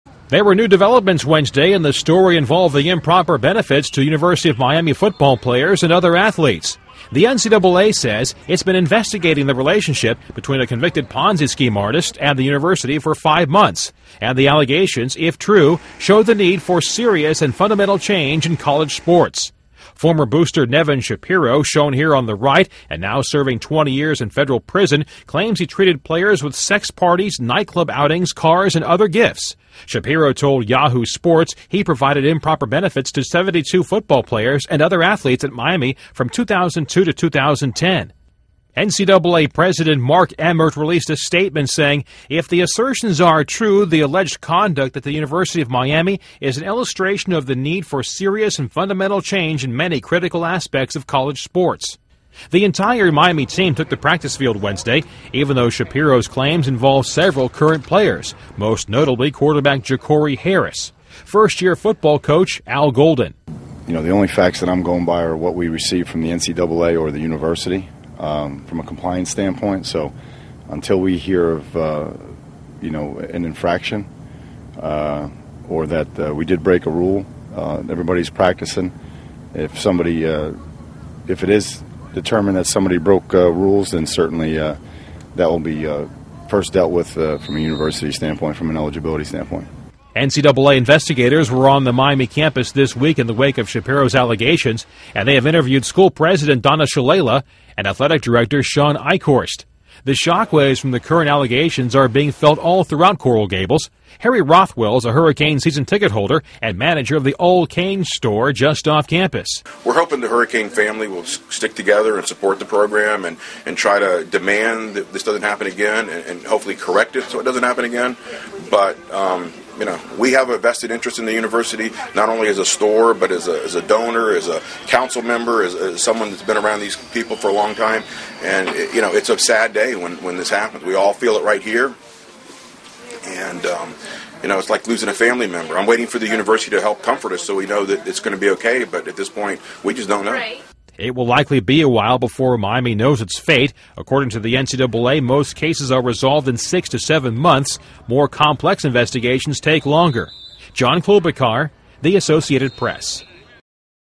NCAA Is 5 Months Into Miami Investigation
The NCAA said Wednesday it has been investigating the relationship between a convicted Ponzi schemer and the University of Miami for five months. (Aug. 17)